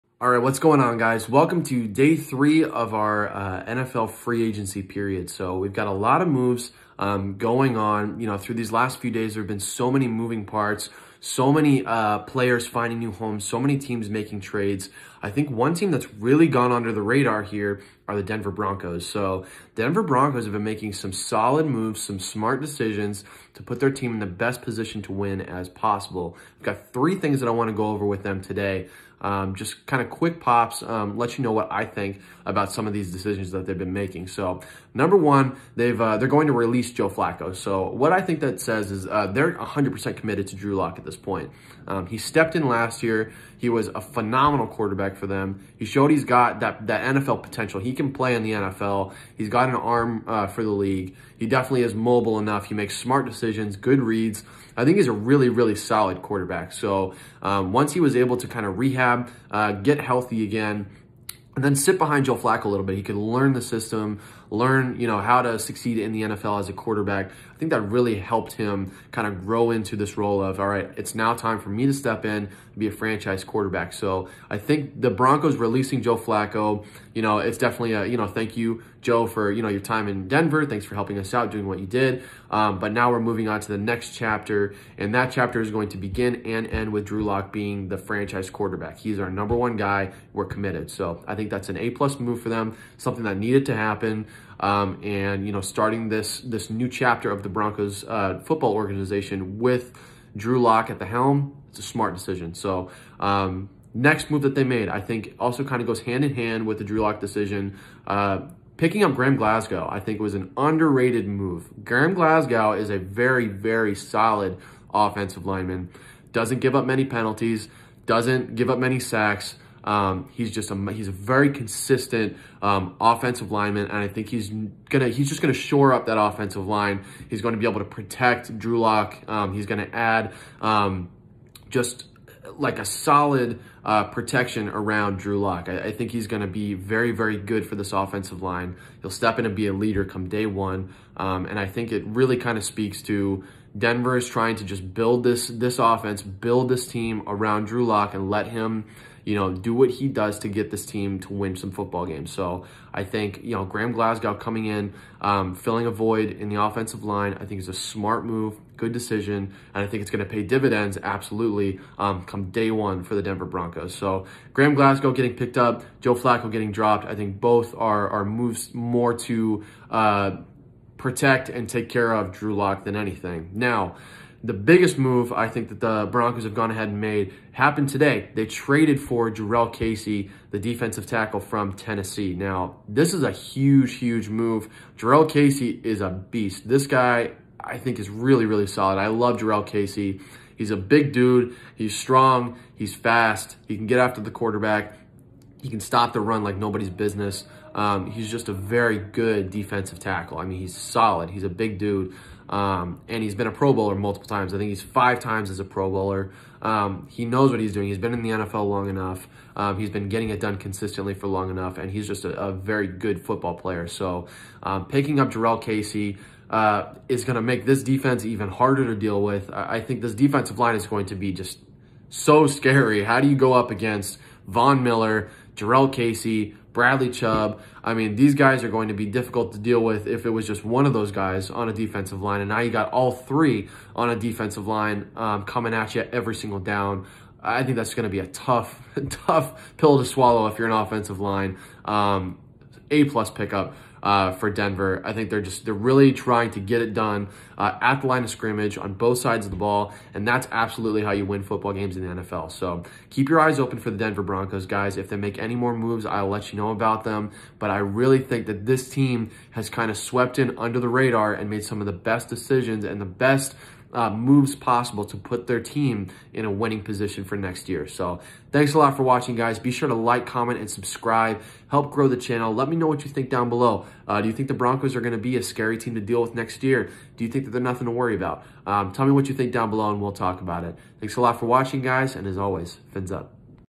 DENVER TRADES FOR JURRELL CASEY! | NFL OFFSEASON 2020
The Denver Broncos have quietly been one of the biggest move-makers in the NFL during free agency. They have picked up key players through free agency, and via trades. Watch to find out who they have picked up!